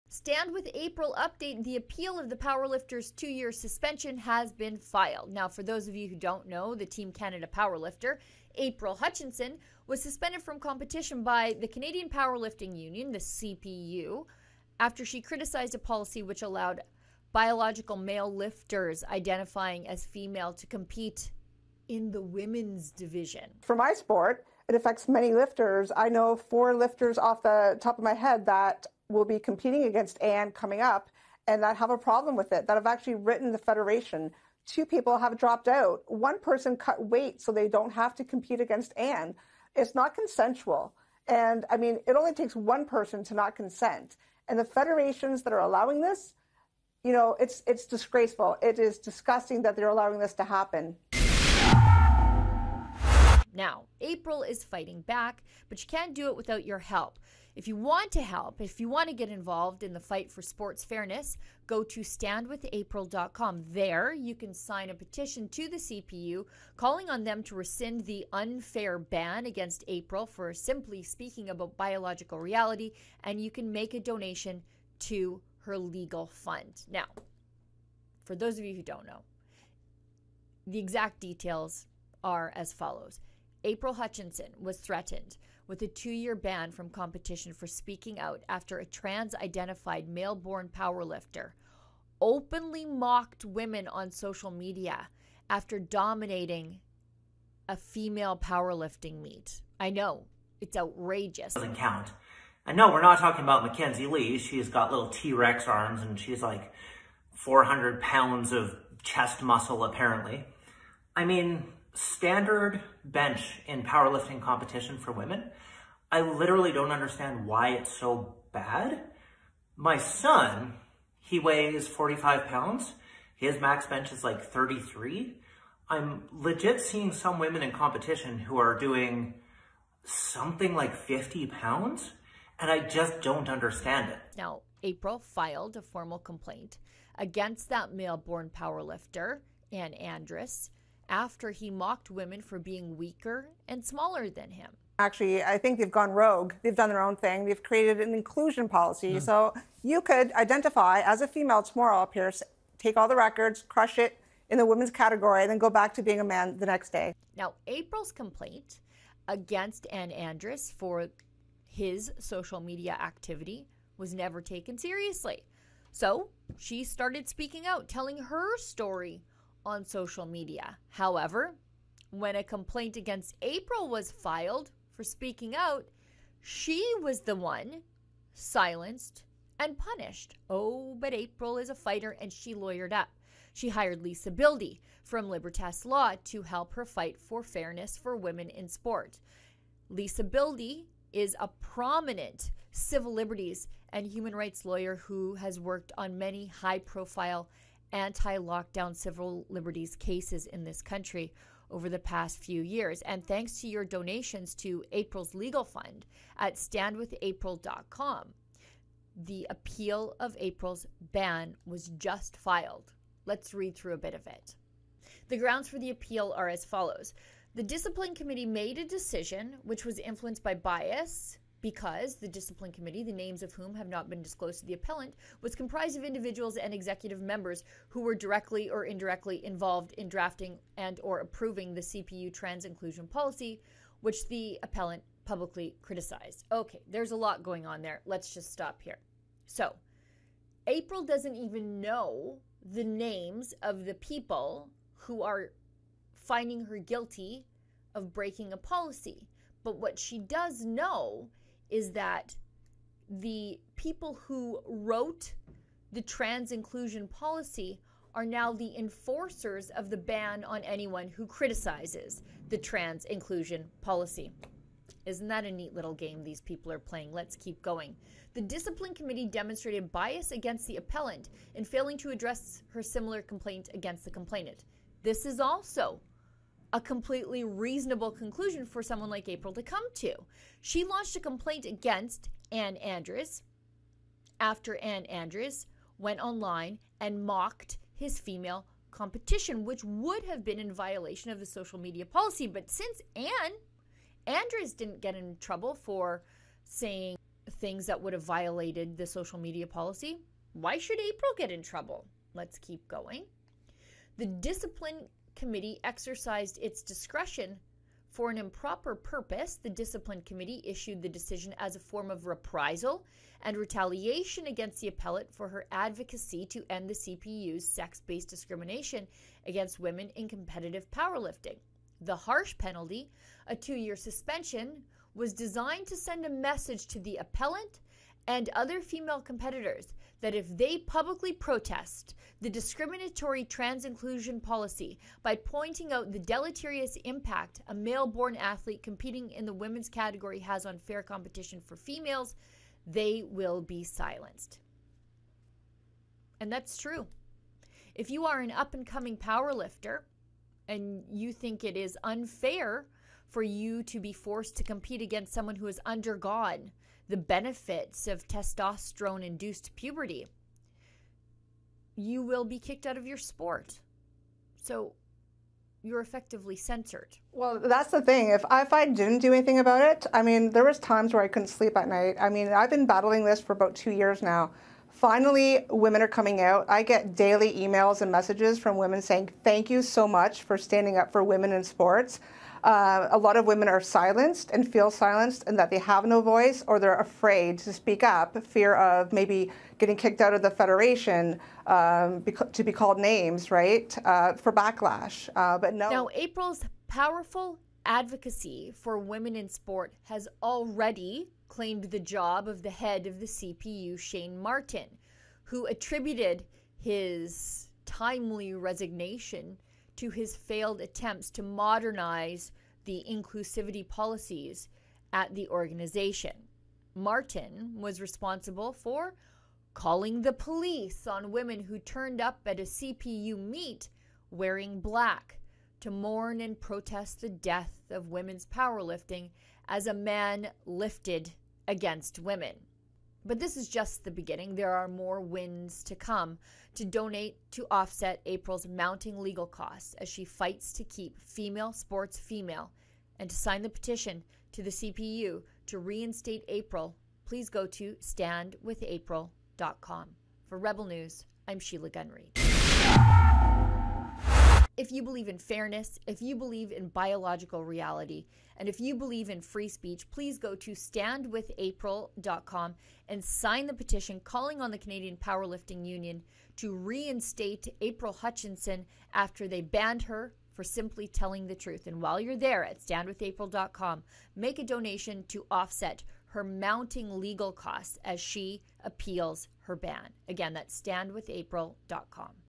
Stand With April UPDATE: Appeal for 2-year suspension filed
| April Hutchinson's powerful advocacy for the women in her sport has claimed the job of the head of the CPU, Shane Martin, who attributed his resignation to his failed attempts to modernize the inclusivity policies of the organization.